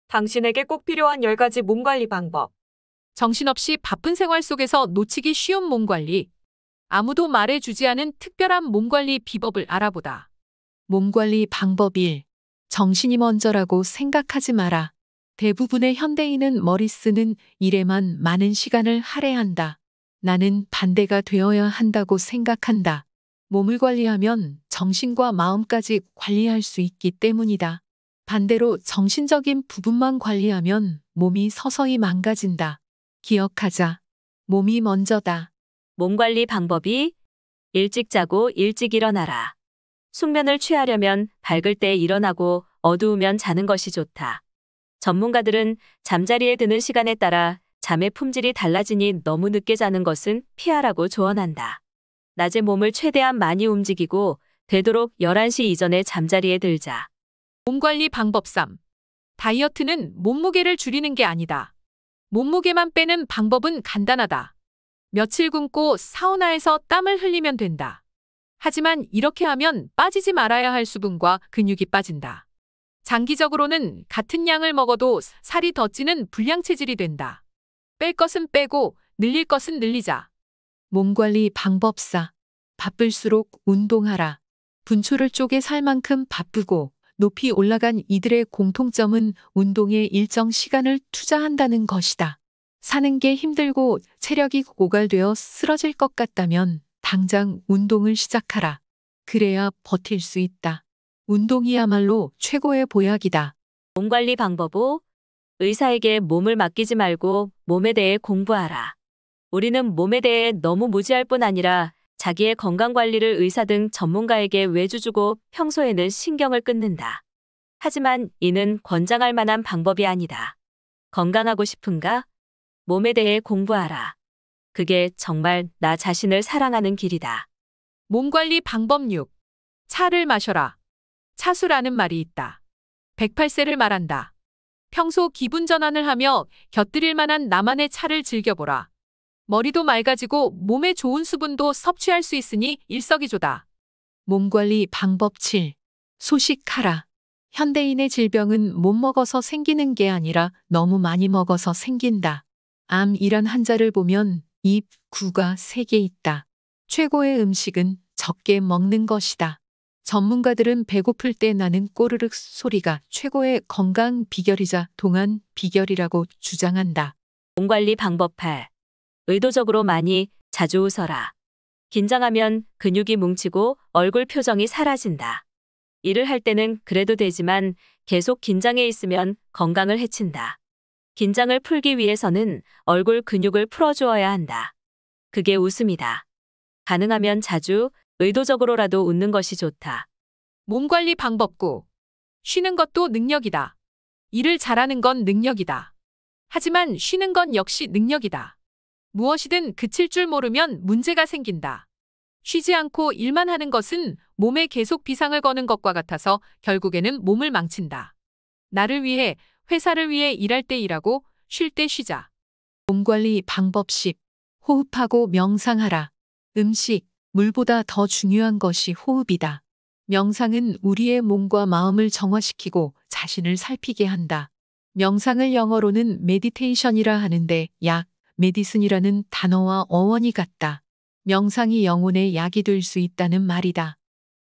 당신에게 꼭 필요한 10가지 몸관리 방법
정신없이 바쁜 생활 속에서 놓치기 쉬운 몸관리. 아무도 말해 주지 않은 특별한 몸관리 비법을 알아본다.
몸관리 방법1. 정신이 먼저라고 생각하지 마라. 대부분의 현대인은 머리 쓰는 일에만 많은 시간을 할애한다. 나는 반대가 되어야 한다고 생각한다. 몸을 관리하면 정신과 마음까지 관리할 수 있기 때문이다. 반대로 정신적인 부분만 관리하면 몸이 서서히 망가진다. 기억하자. 몸이 먼저다.
몸관리 방법2. 일찍 자고 일찍 일어나라. 숙면을 취하려면 밝을 때 일어나고 어두우면 자는 것이 좋다. 전문가들은 잠자리에 드는 시간에 따라 잠의 품질이 달라지니 너무 늦게 자는 것은 피하라고 조언한다. 낮에 몸을 최대한 많이 움직이고, 되도록 11시 이전에 잠자리에 들자.
몸관리 방법3. 다이어트는 몸무게를 줄이는 게 아니다. 몸무게만 빼는 방법은 간단하다. 며칠 굶고 사우나에서 땀을 흘리면 된다. 하지만 이렇게 하면 빠지지 말아야 할 수분과 근육이 빠진다. 장기적으로는 같은 양을 먹어도 살이 더 찌는 ‘불량체질’이 된다. 뺄 것은 빼고, 늘릴 것은 늘리자.
몸관리 방법4. 바쁠수록 운동하라. 분초를 쪼개 살 만큼 바쁘고, 높이 올라간 이들의 공통점은 운동에 일정 시간을 투자한다는 것이다. 사는 게 힘들고 체력이 고갈되어 쓰러질 것 같다면 당장 운동을 시작하라. 그래야 버틸 수 있다. 운동이야말로 최고의 보약이다.
몸관리 방법5. 의사에게 몸을 맡기지 말고 몸에 대해 공부하라. 우리는 몸에 대해 너무 무지할 뿐 아니라 자기의 건강 관리를 의사 등 전문가에게 외주 주고 평소에는 신경을 끊는다. 하지만 이는 권장할만한 방법이 아니다. 건강하고 싶은가? 몸에 대해 공부하라. 그게 정말 나 자신을 사랑하는 길이다.
몸관리 방법6. 차(茶)를 마셔라. ‘차수(茶壽)’라는 말이 있다. 108세를 말한다. 평소 기분전환을 하며 곁들일만한 나만의 차를 즐겨보라. 머리도 맑아지고 몸에 좋은 수분도 섭취할 수 있으니 일석이조다.
몸관리 방법7. 소식(小食)하라. 현대인의 질병은 못 먹어서 생기는 게 아니라 너무 많이 먹어서 생긴다. ‘암(癌)’이란 한자를 보면 ‘입 구(口)’가 세 개 있다. 최고의 음식은 적게 먹는 것이다. 전문가들은 배고플 때 나는 꼬르륵 소리가 최고의 건강 비결이자 동안 비결이라고 주장한다.
몸관리 방법8. 의도적으로 많이, 자주 웃어라. 긴장하면 근육이 뭉치고 얼굴 표정이 사라진다. 일을 할 때는 그래도 되지만 계속 긴장해 있으면 건강을 해친다. 긴장을 풀기 위해서는 얼굴 근육을 풀어주어야 한다. 그게 웃음이다. 가능하면 자주, 의도적으로라도 웃는 것이 좋다.
몸관리 방법9. 쉬는 것도 능력이다. 일을 잘하는 건 능력이다. 하지만 쉬는 것 역시 능력이다. 무엇이든 그칠 줄 모르면 문제가 생긴다. 쉬지 않고 일만 하는 것은 몸에 계속 비상을 거는 것과 같아서 결국에는 몸을 망친다. 나를 위해, 회사를 위해 일할 때 일하고, 쉴 때 쉬자.
몸관리 방법10. 호흡하고 명상하라. 음식, 물보다 더 중요한 것이 호흡이다. 명상은 우리의 몸과 마음을 정화시키고 자신을 살피게 한다. 명상을 영어로는 ‘메디테이션’이라 하는데 ’약, 메디슨’이라는 단어와 어원이 같다. 명상이 영혼에 약이 될 수 있다는 말이다.